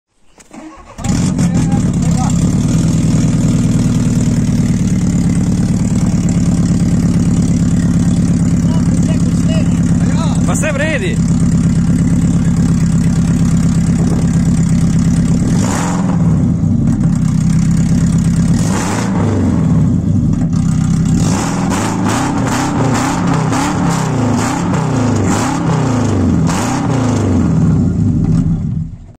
1.6 boxer with no exhaust.
Exhaust is removed for rear subframe refurbish. We just had to film this hellish sounds because I will probably never hear it again.

Enjoy the devil.